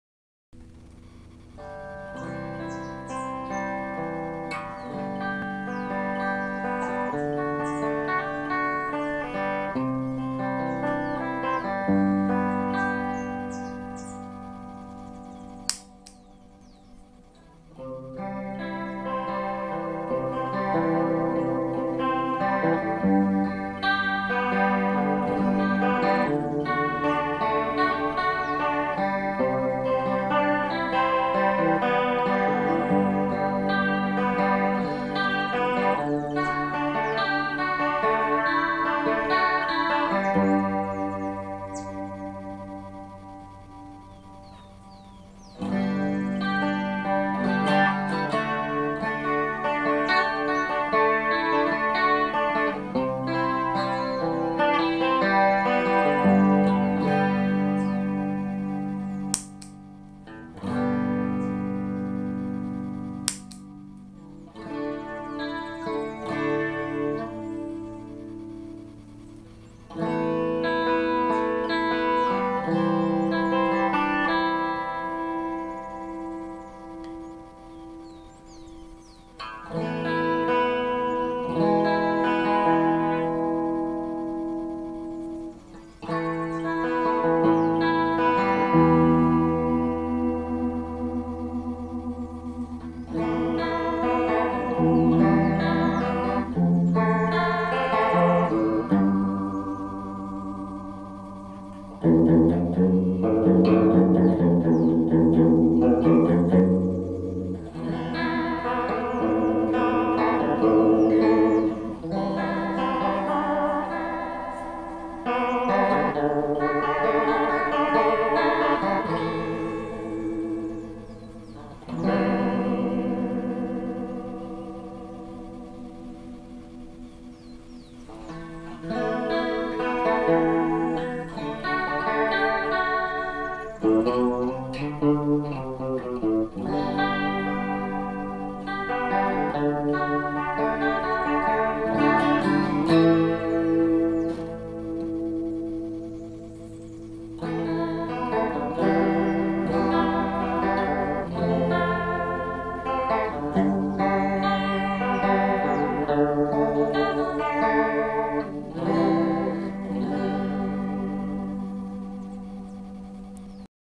MURBACH EFFECTS CE-2 Clone
Guitarra Tagima T-635 com Seymour Duncan
Amplificador Borne Clássico T7 valvulado.